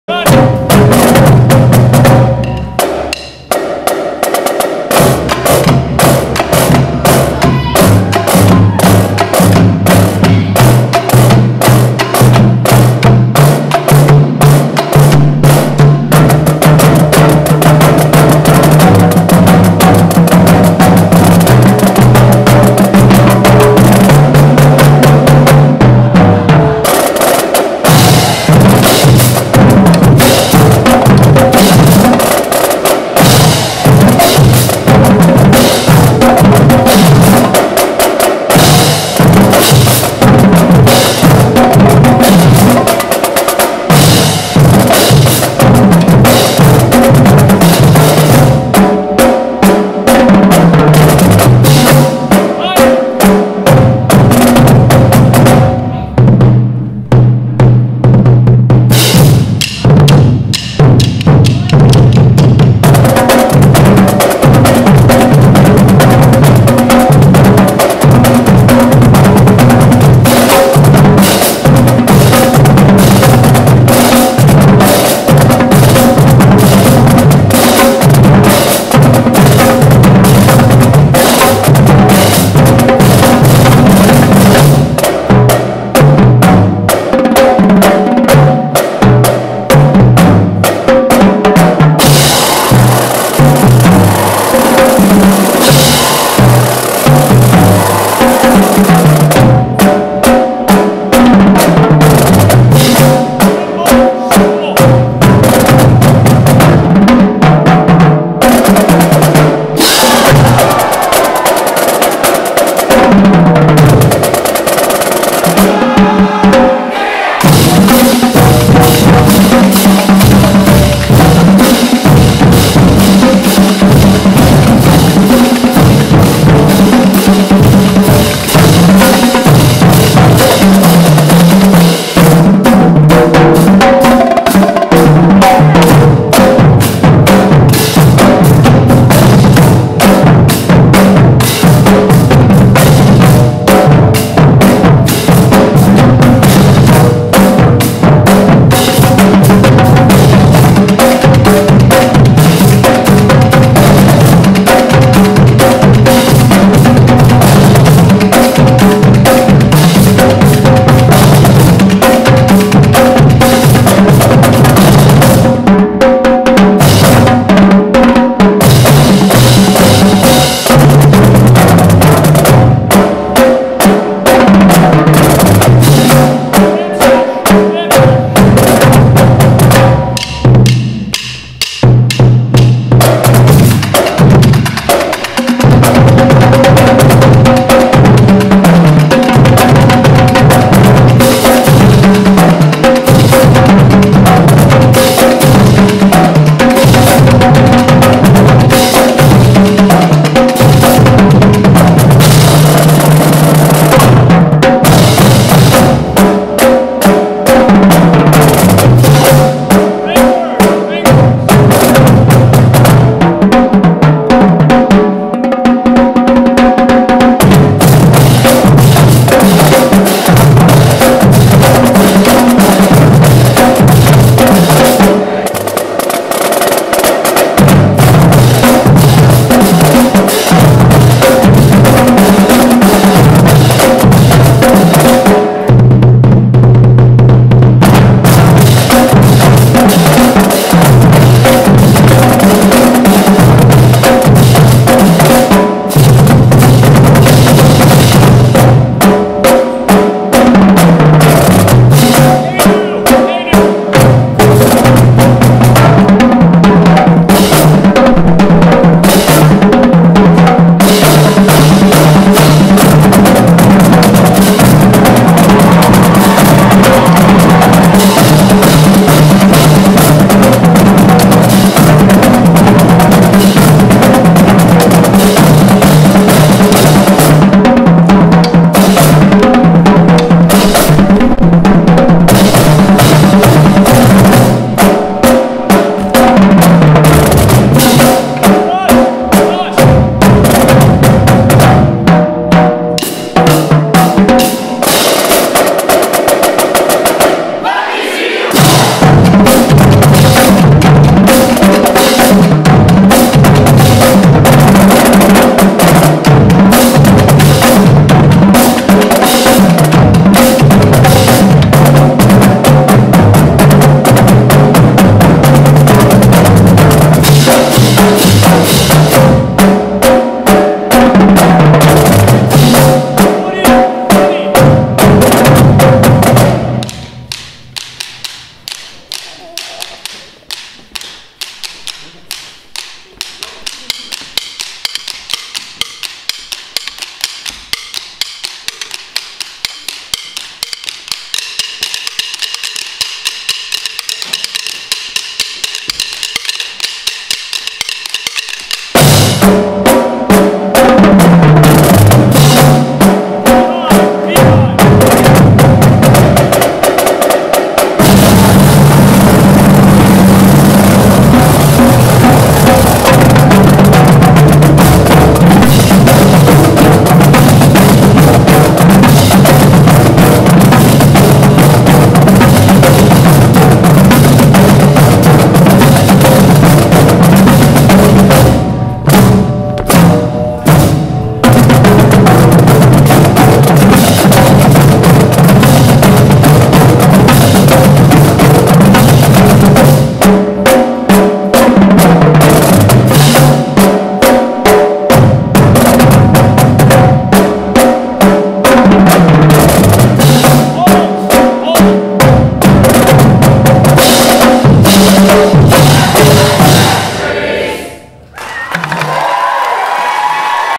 10 25 2017 Drum Line
Drum Line performs at BHS concert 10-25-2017